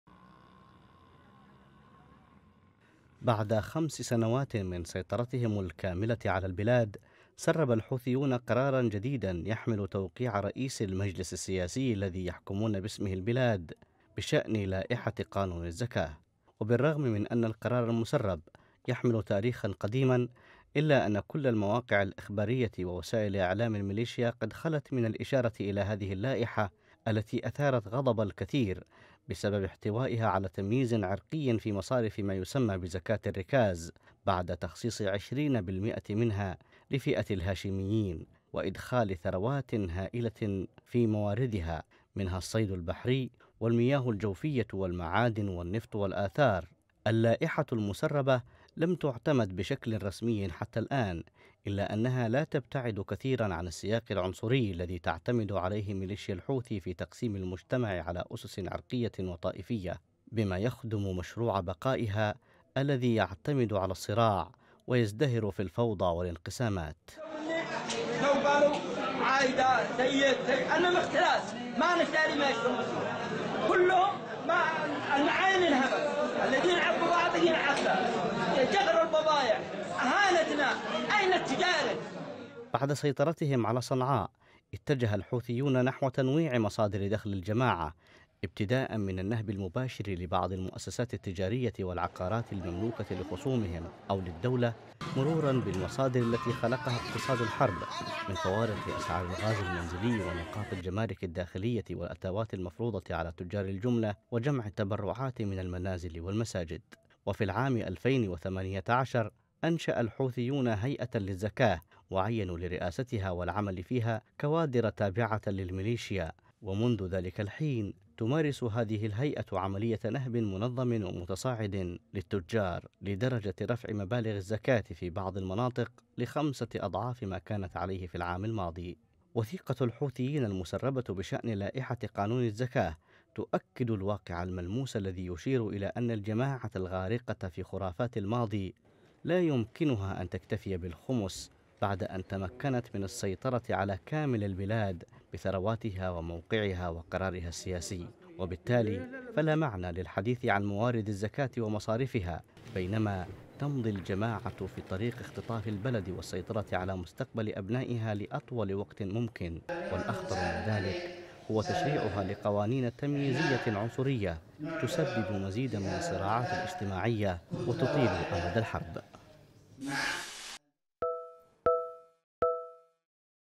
لائحة قانون الزكاة.. مفتاح حوثي جديد لسرقة أموال اليمنيين | تقرير: موسى النمراني
إجراءٌ جديدٌ لمليشيا الحوثي يقرر "الخمس" لبنى هاشم من ثرواتِ البلاد
فهل الخمس هذا جزء من التشريع أو النص الديني؟ أم أنه تأكيدُ الأفضلية الهاشمية على اليمنيين ؟ ماخطورةُ الفرز المجتمعي وما موقعُ هذه الإشكاليات في الدستور ومخرجاتِ مؤتمرِ الحوار؟